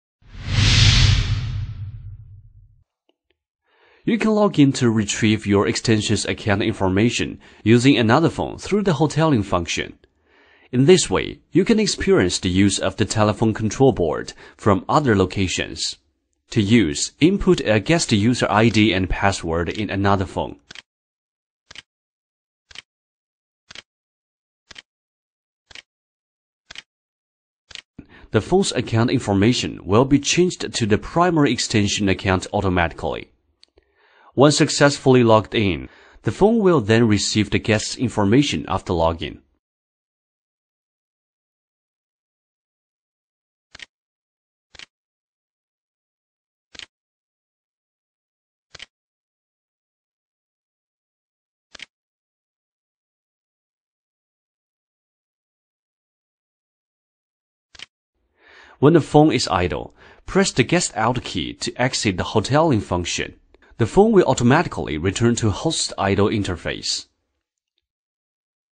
T46 BroadSoft Hoteling
You can log in to retrieve your extension's account info using another phone through the hoteling function